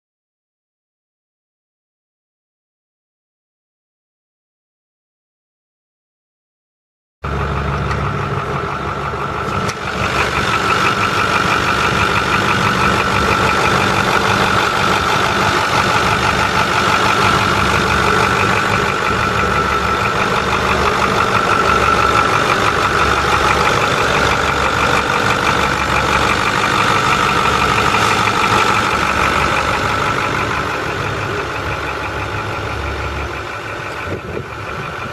2009 Ford F250 Super Duty pickup truck for sale | no-reserve Internet auction March 21, 2017
2009 Ford F250 Super Duty pickup truck
205,663 miles on odometer
6.4L V8 OHV 32V turbo diesel engine
Automatic transmission
AC and heat
Tilt
Cruise
AM/FM
Manual windows and locks
Vinyl interior
Front tow hooks
(3) toolboxes
Auxiliary fuel tank
80 gallon capacity
Electric transfer pump and meter
Receiver hitch
Trailer wiring
245/75R17 tires
Check engine light on
Engine runs rough intermittently
1FTSF20R99EB05735
It's located in Norton, KS, and we're selling it to the highest bidder regardless of price.